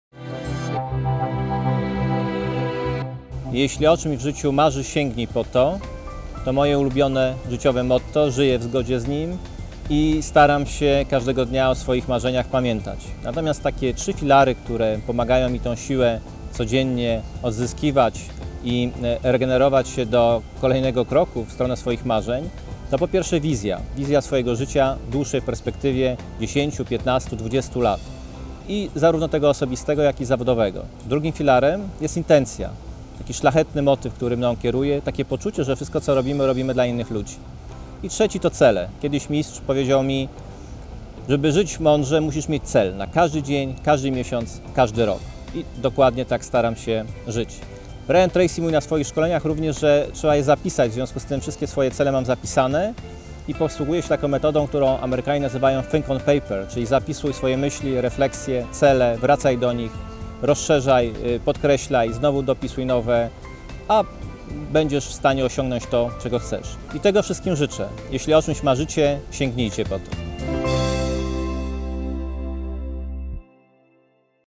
Kilka słów od... - Jacek Walkiewicz | WSB w Chorzowie
,,Jeśli o czymś w życiu marzysz, sięgnij po to" - to ulubione motto Jacka Walkiewicza, psychologa i członka Stowarzyszenia Profesjonalnych Mówców. Posłuchaj, co pomaga mu w osiąganiu celów.

Jacek Walkiewicz był gościem specjalnym XVI Forum Praktyki Gospodarczej w Wyższej Szkole Bankowej w Chorzowie.